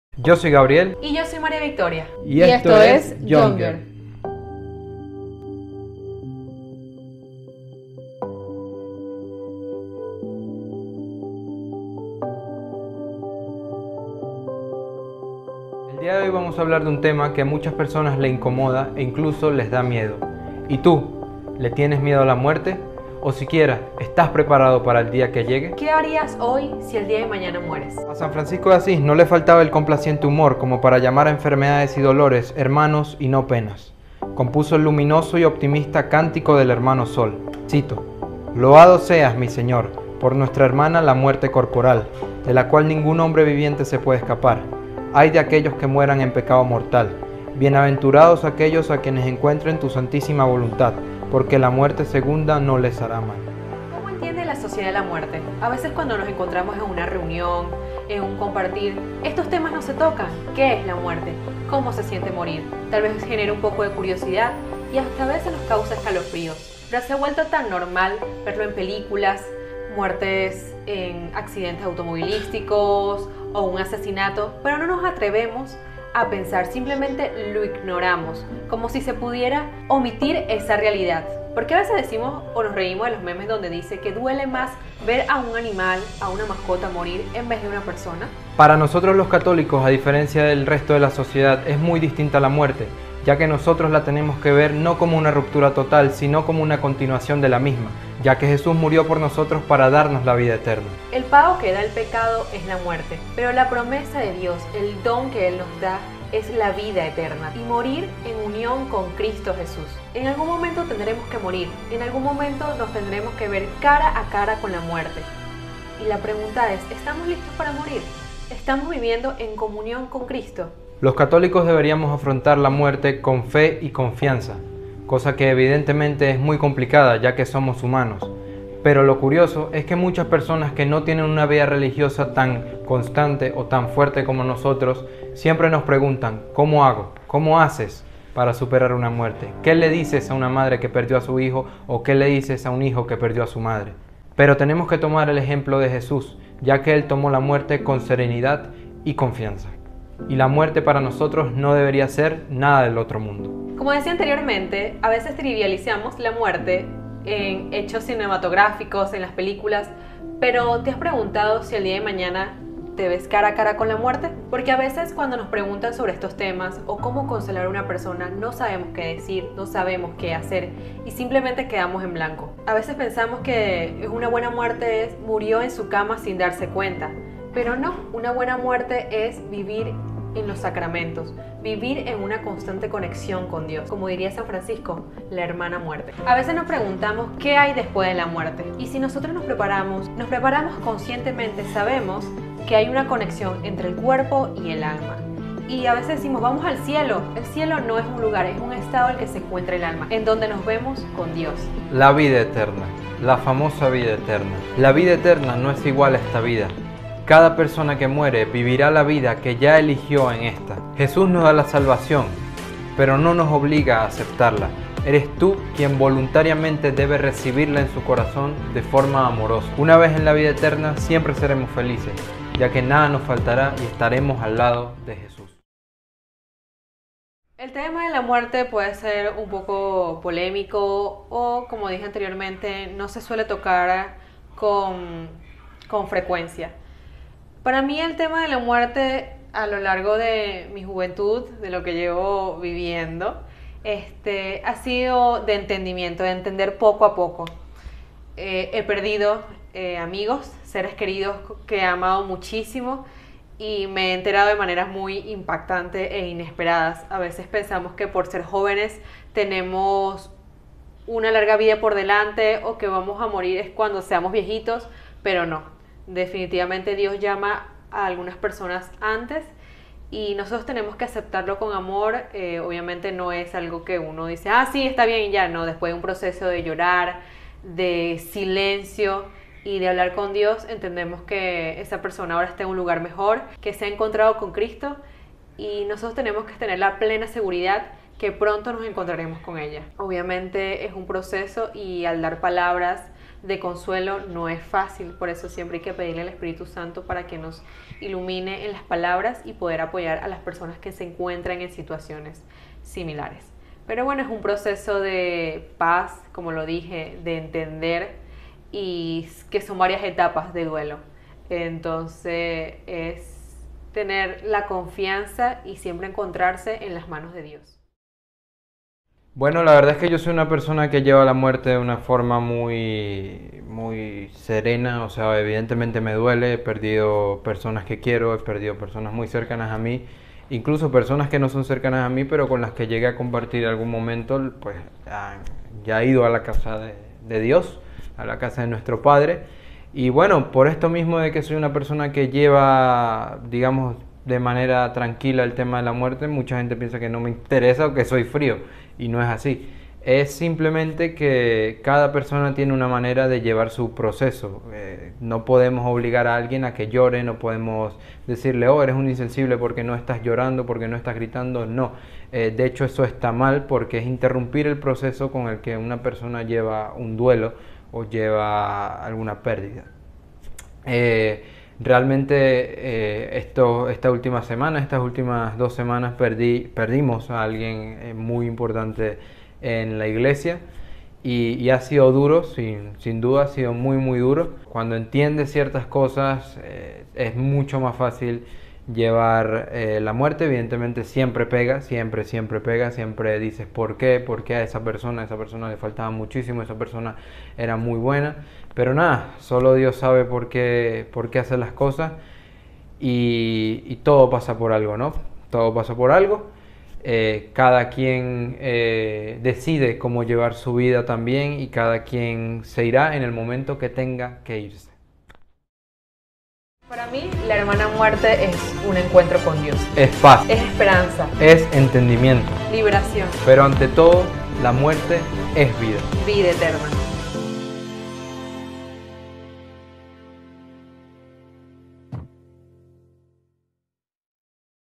¿Le tienes miedo a la muerte?
¿Temes a la muerte?¿Vives un duelo?
La muerte es una experiencia natural en el desarrollo de cada ser humano. Algunos hemos sentido el dolor de perder a un ser querido, y otros lo experimentaran en algún momento.
Hoy hablaremos de nuestra ExperienciaConLaMuerte, sobre todo, contaremos como nos fue con está realidad que San Francisco llamo HermanaMuerte.